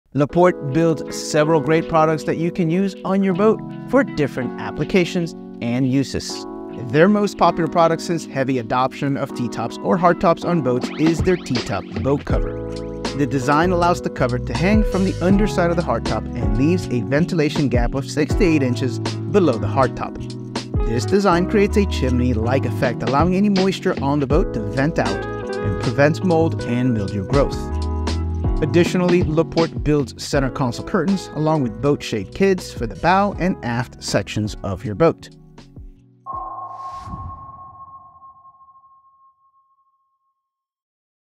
EPISODE 6 COMING FRIDAY - Building The Industry Leading Boat Cover - Sportsman's "Behind The Glass"
In this exciting episode of Behind The Glass, we’re taking a short trip from the Sportsman Boats factory to Charleston, South Carolina, to visit LaPorte Products. LaPorte, a family-owned company with over 35 years of experience, specializes in crafting premium boat covers and marine products. Join us as we bring along the brand-new Open 212 and dive into LaPorte’s innovative process, from CAD modeling to CNC precision cutting, to see how they create their custom-fit covers.

We’ll explore the design and manufacturing steps, including how LaPorte builds their famous T-top boat covers and shade kits that ensure your boat stays protected. With cutting-edge technology and a deep-rooted focus on craftsmanship, LaPorte Products continues to deliver high-quality covers that meet the rigorous standards of Sportsman Boats.

Tune in this Friday for Episode 6, and don’t miss another exciting chapter as we continue to showcase the innovation and passion behind Sportsman Boats.